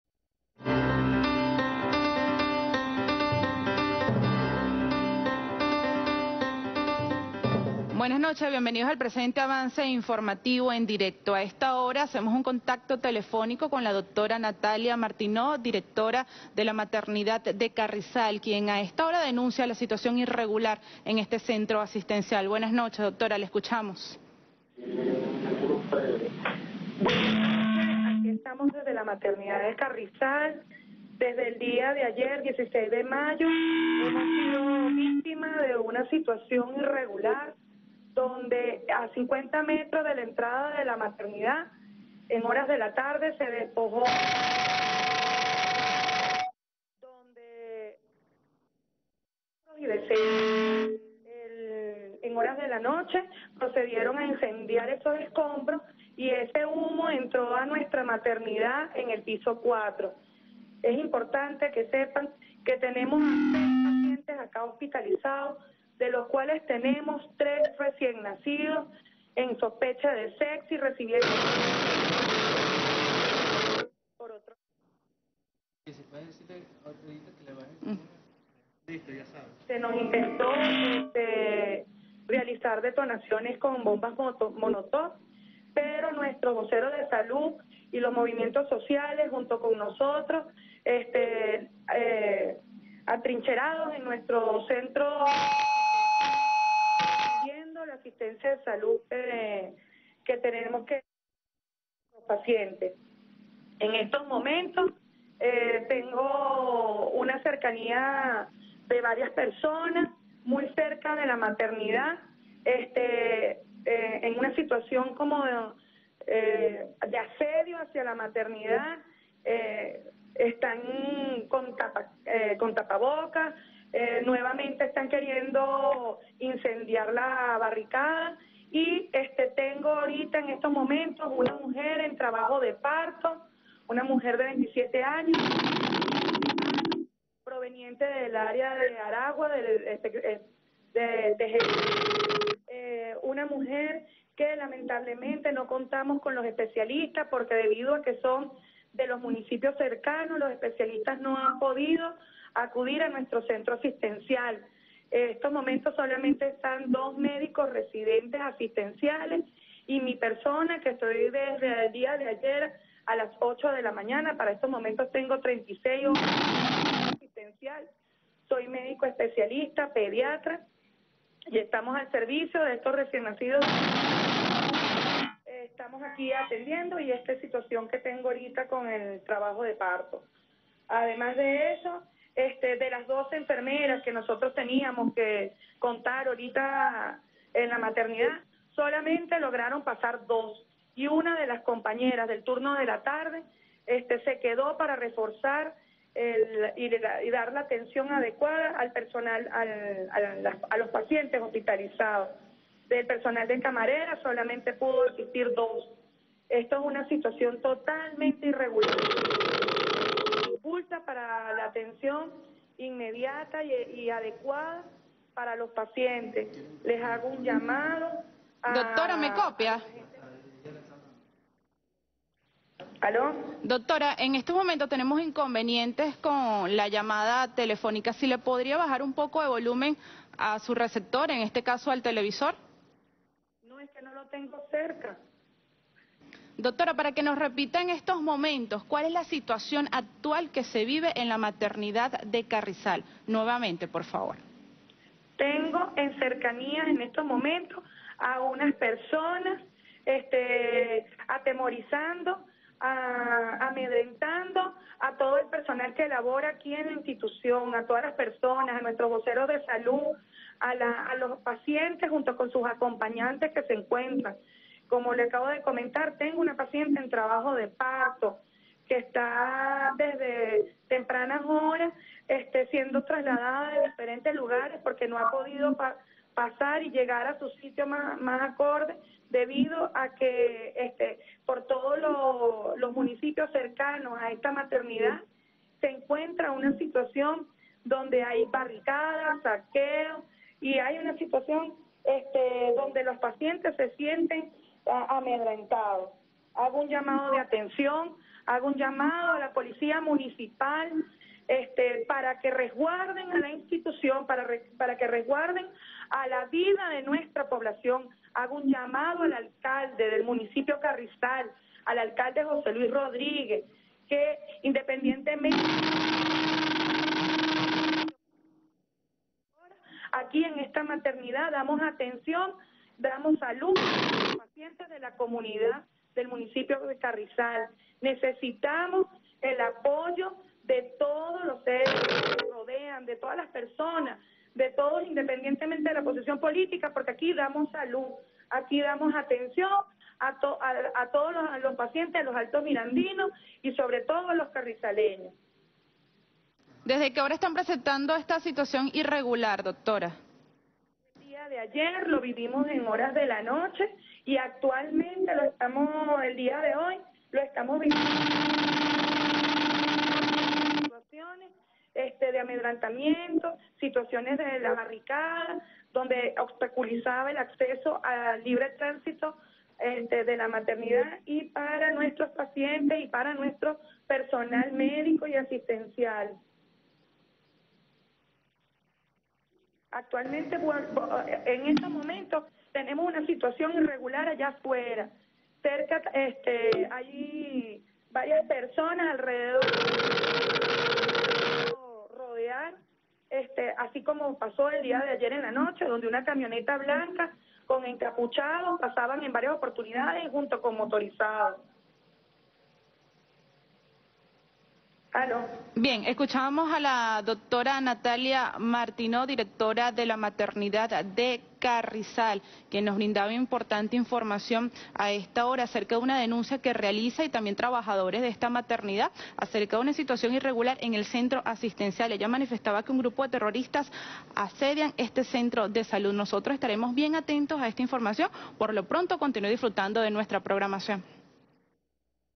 Dra. Natalia Martinho denuncia grave asedio a Maternidad de Carrizal
(FALLAS DE ORIGEN) La directora de la Maternidad de Carrizal, estado Miranda, denuncia dos días de asedio por parte de guarimberos y personas violentas, que impiden el paso y la llegada de médicos y personal y queman escombros causando afecciones a los pacientes y parturientas allí hospitalizadas. Situación GRAVE.